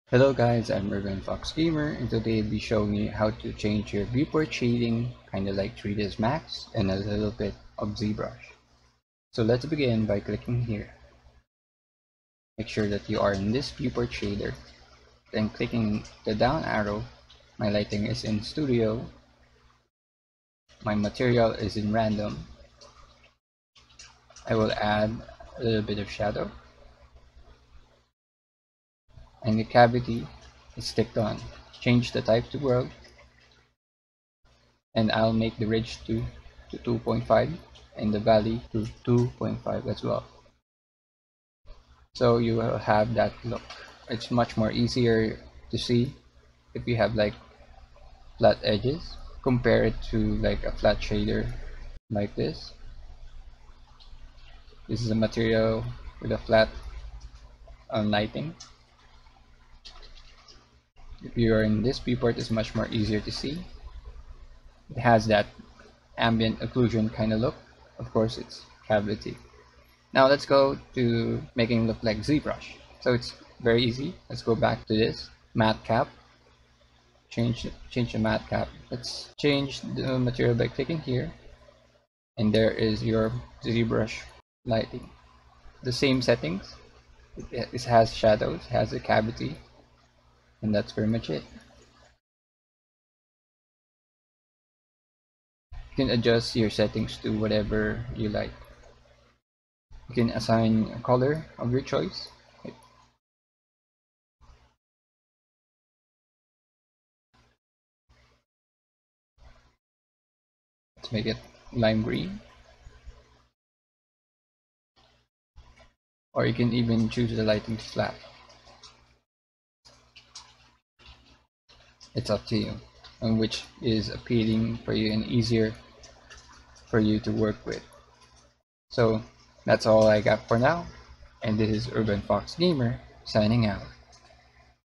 How to make Blender look like Zbrush
a little tutorial that i just wanted to make cuz of boredom and a platform to thank Imphezia for Inspiring me to start my blender tutorial series and my new journey as blender user.